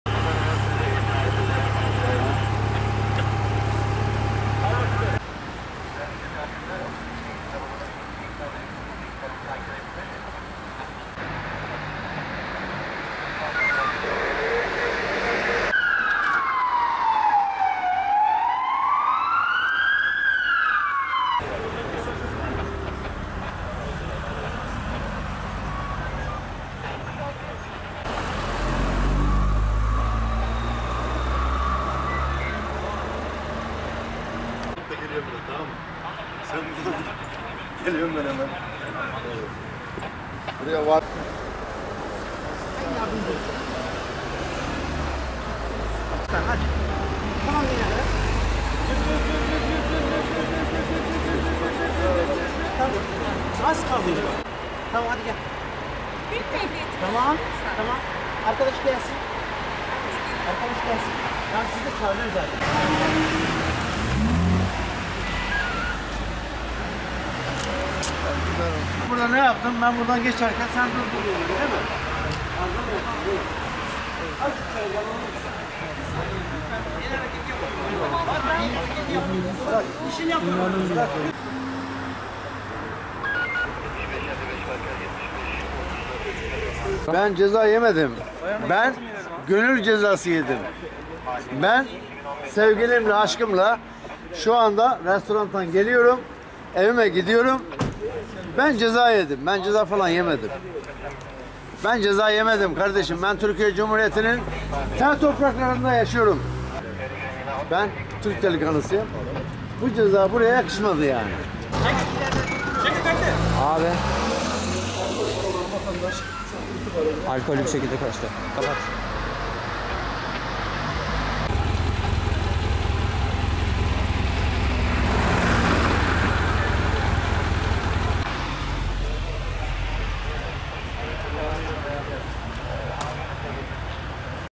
Alkollü araç kullanırken yakalandı ‘Ben Türk delikanlısıyım’ dedi
Denizli’de alkollü olarak araç kullanırken yakalanan sürücü ceza yazıldıktan sonra polislerin işlem yaptıkları esnada arabasına binerek kaçtı. Ekipler kaçan sürücüyü yakalamak için çalışma başlattı. Sürücü kaçmadan önce ise İHA muhabirinin ‘ne kadar ceza yediniz’ sorusuna ise “Ben ceza yemedim ben gönül cezası yedim. Ben Türk delikanlısıyım bu ceza buraya yakışmaz” diye yanıtladı.